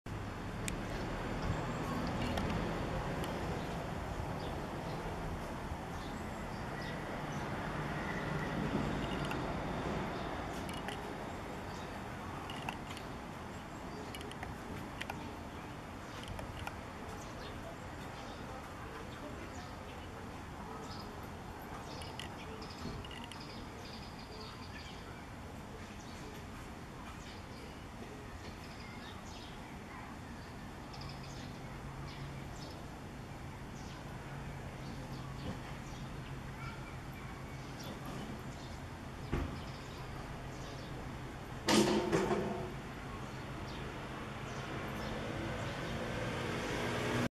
帰ってきたモズ PowerShot SX50 HS
９月中旬にモズ子が帰ってきました。
ここ数日、鳴きまくっていて、初めて姿が見えました。
最後はデジタルズームで１００倍。